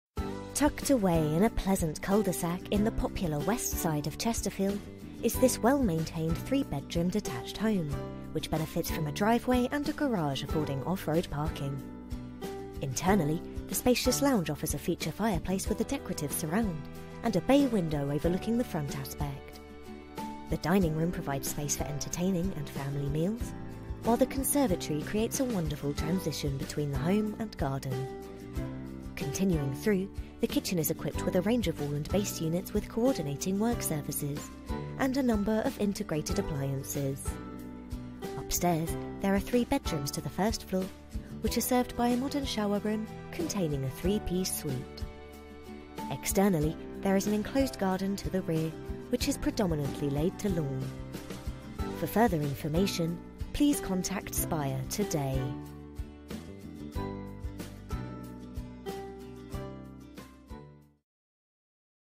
4 Delves close
A WELL MAINTAINED three bedroom detached house, tucked away in a pleasant CUL-DE-SAC, in a POPULAR AREA to the west of town. Offered with NO UPWARD CHAIN.
Benefiting from having block paved driveway parking for 2-3 cars, an attached LARGER THAN AVERAGE GARAGE and an enclosed rear garden with patio and lawn.
This EXCELLENT FAMILY HOME comprises: - entrance hall, spacious lounge with bay window, separate dining room, fitted kitchen with some integrated appliances, conservatory, three first floor bedrooms and modern combined shower room/wc.
The property is gas centrally heated (combi) and uPVC double glazed - including soffits/fascias.
Situated close to local amenities. A viewing is recommended.